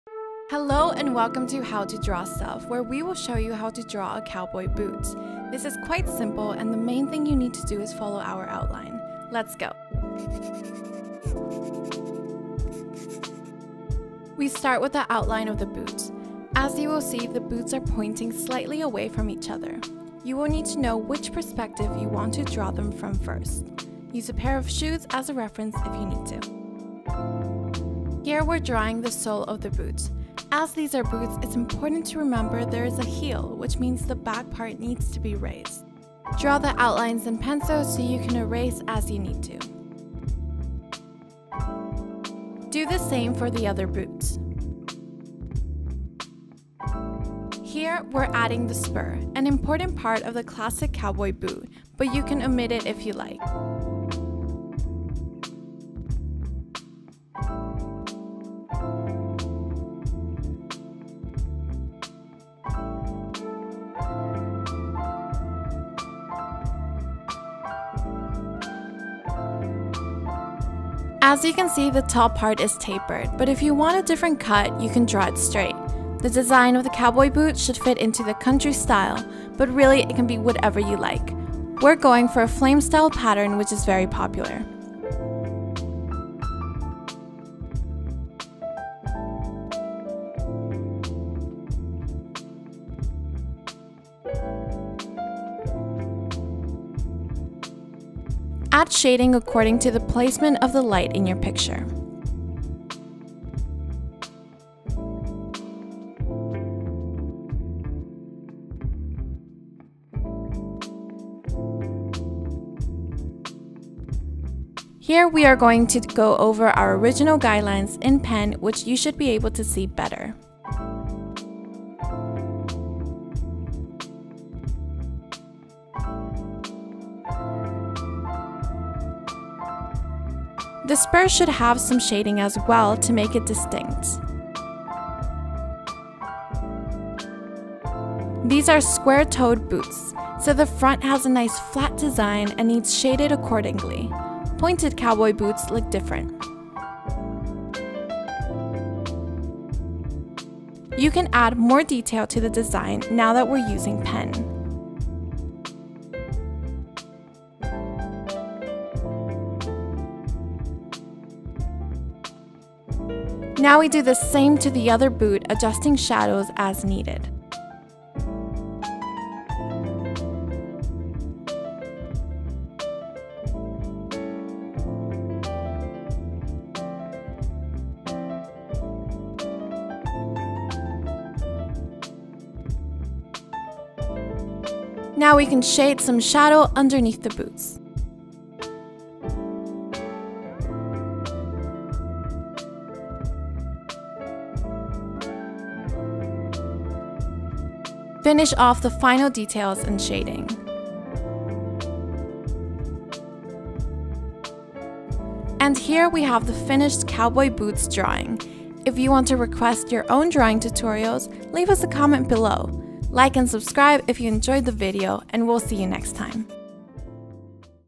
How to Draw Cowboy Boots
If you want to know how to draw cowboy boots for an illustration or whatever design, our
How to Draw Stuff tutorial has all you need.